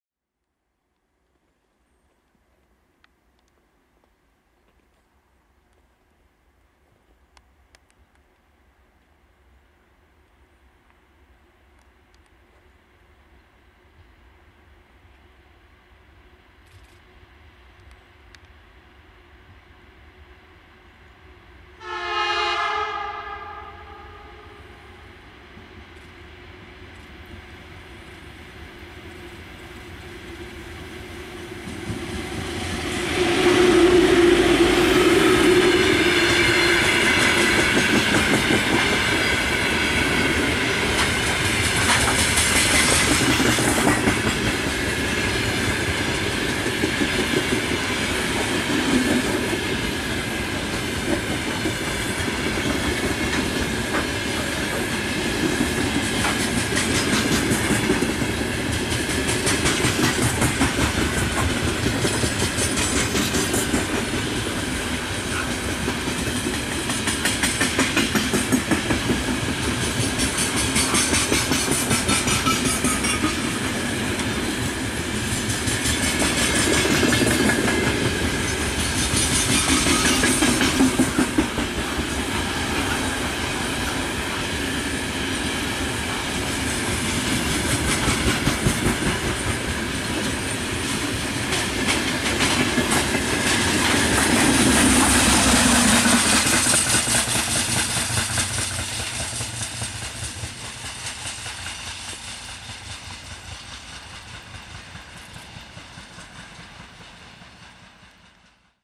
"Тифон как от грузовика" Электровоз ВЛ10У-609 с грузовым поездом
ВЛ10У-609 - утяжелённый электровоз, колёса которого имеют большую силу сцепления с рельсами, благодаря чему он способен возить более тяжёлые составы с грузовым поездом.